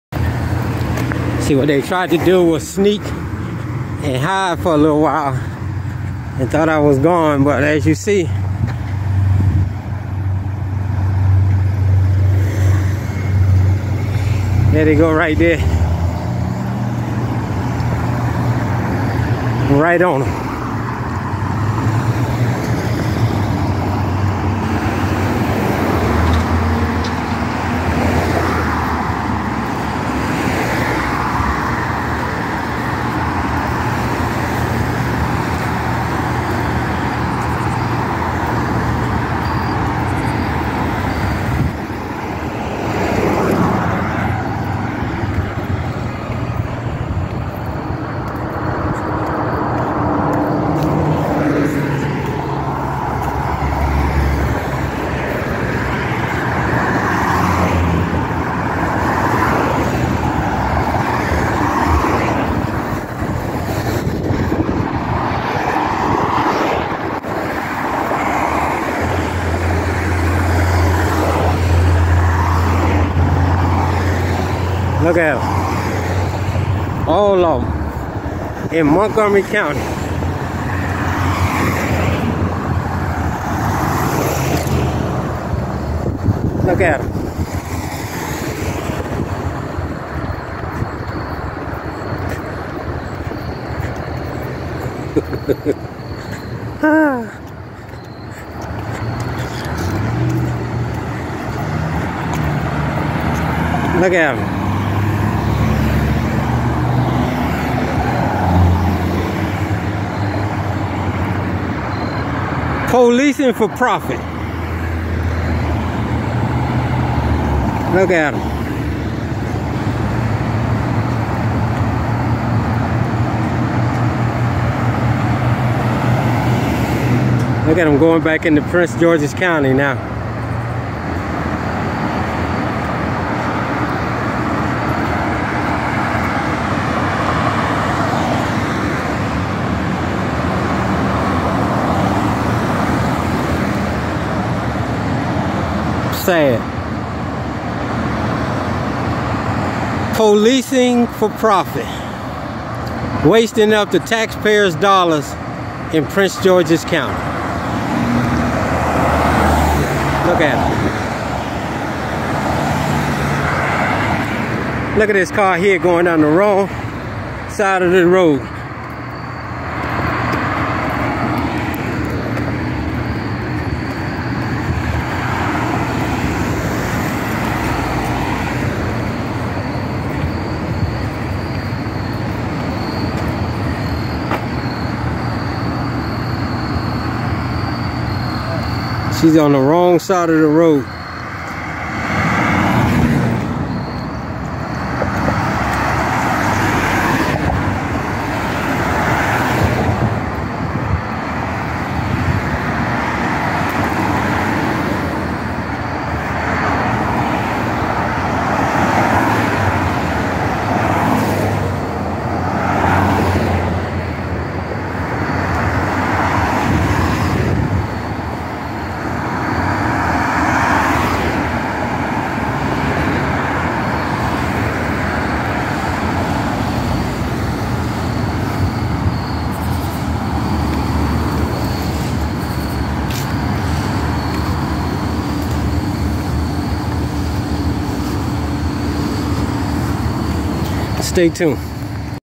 Prince George's County, Maryland Harassing Citizens On Highway 650
October 7th 2022, camera footage, Part  : Prince George's County, Maryland Law Enforcement Officers enter into Montgomery County, Maryland and harass citizen on Highway 650